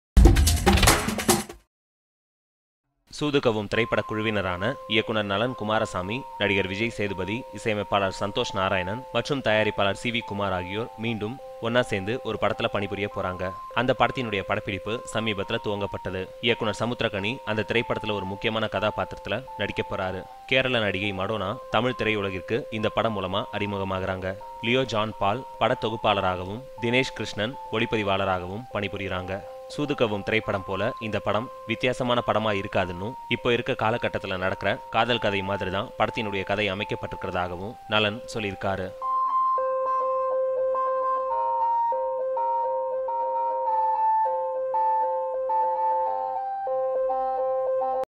Nalan & Vijay Sethupathy's 2nd Outing
Nalan & Vijay Sethupathy's 2nd Outing has madonna in the lead and Santosh narayanan as music director.